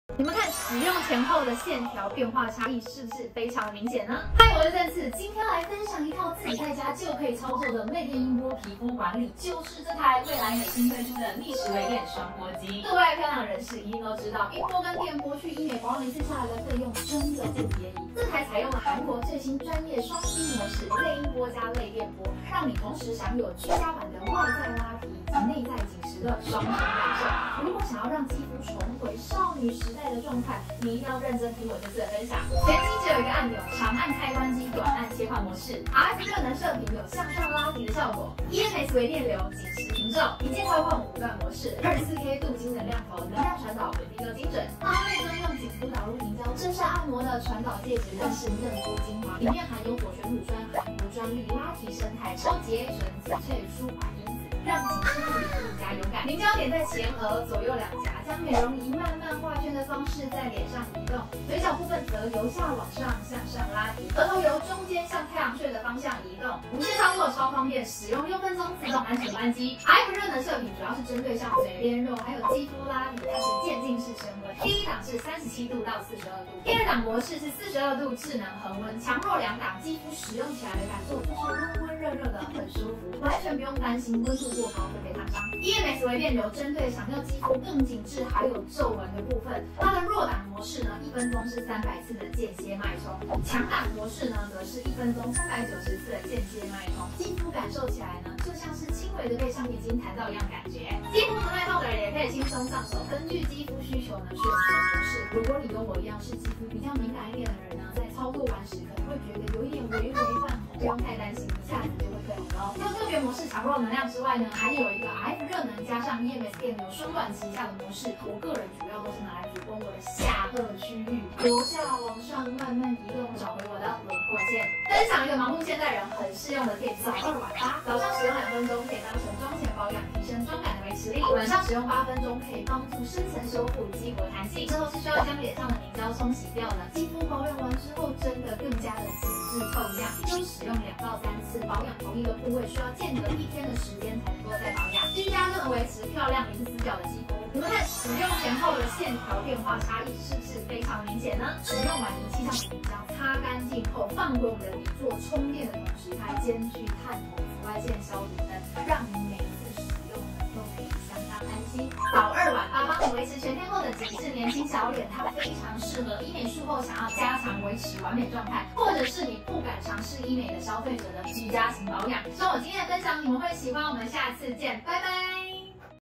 韓過最新!!居家版類電音波美容儀（一機兩種模式get）feat. 未來美
（音波＋電波）去外面做真的不便宜
這台一機就享有（RF熱能射頻＋EMS微電流）
兩種模式一次可以同時擁有❤️
全機簡單一鍵操控5段模式和開關機
24K鍍金能量頭搭配專用導入凝膠
按摩的同時還可以一起保養唷～

早2晚8 幫妳維持全天候維持緊緻年輕小臉
非常適合醫美術後想要加強維持完美狀態
或是不敢嘗試醫美的消費者的居家型保養唷😊